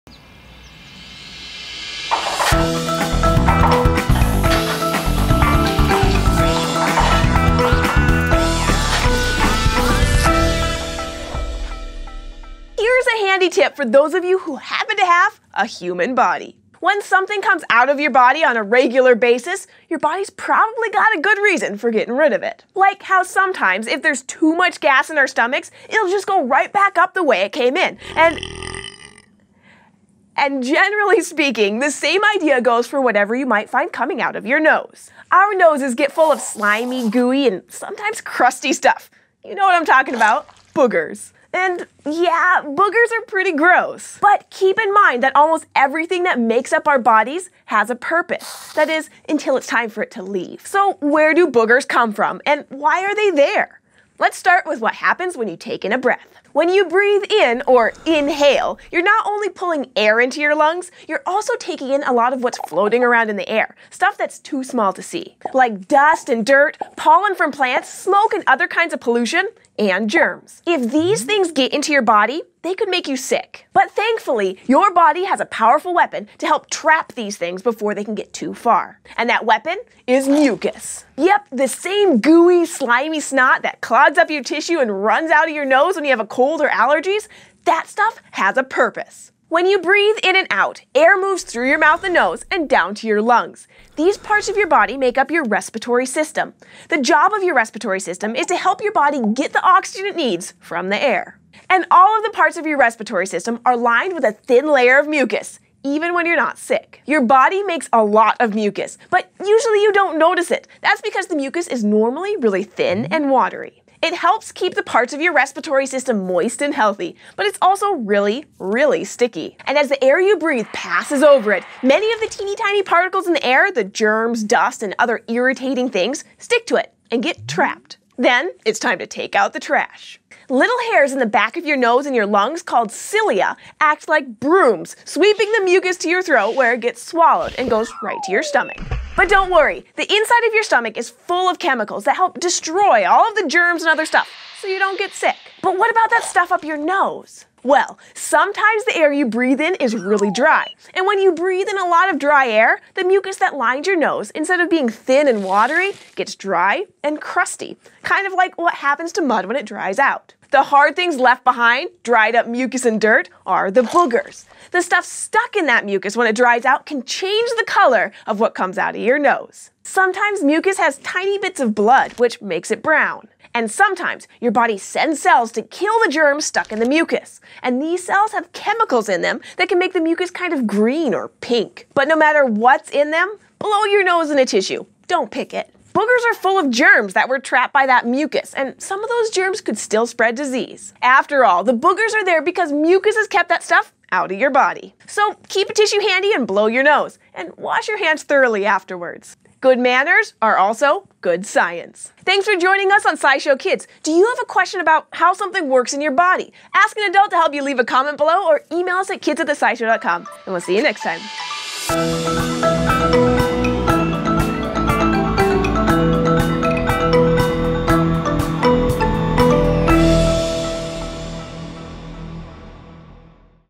What are Boogers?
What’s that stuff up your nose? Does it do anything? SciShow Kids explains the science of boogers!